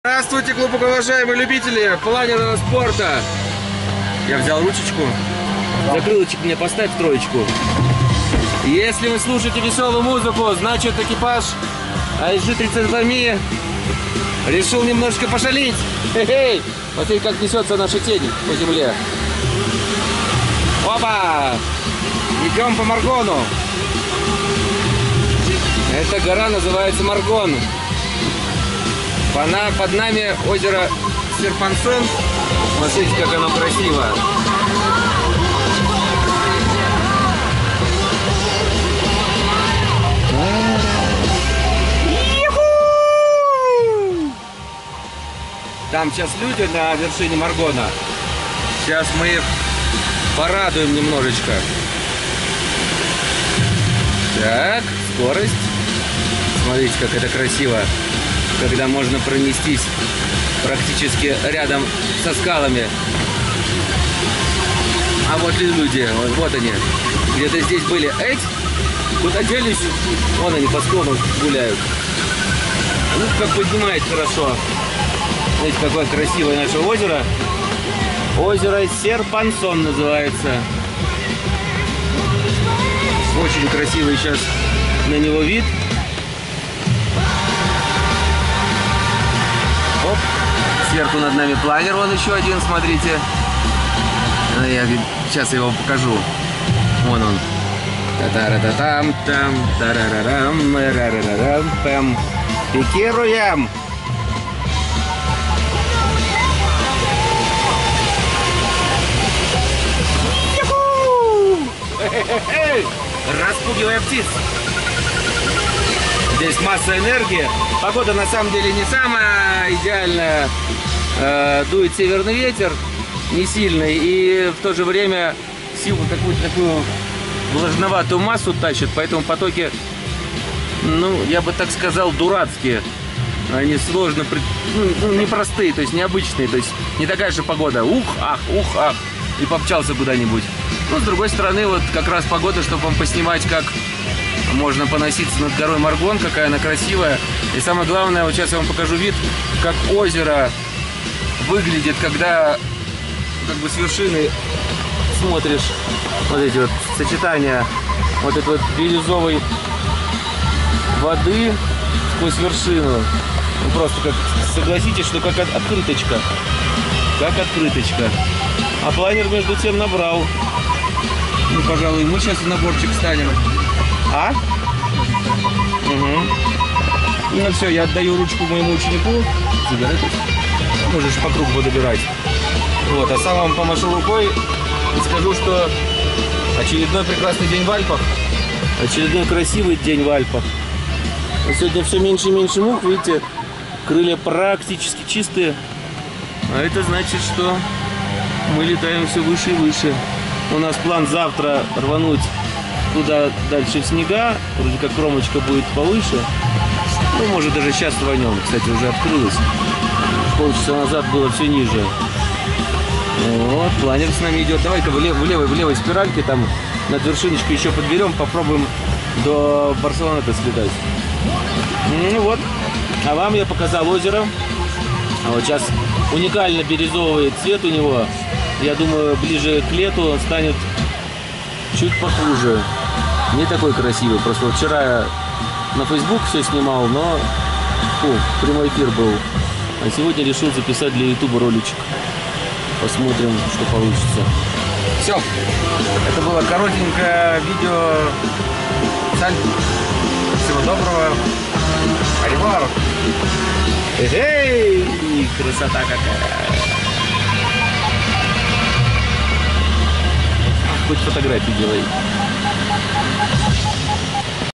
Моргон, ты прекрасен!
Веселое и позитивное видео полетов над озером Серпансон. Планер - это свобода над пространством.

Ваш небесный лектор
Игорь Волков.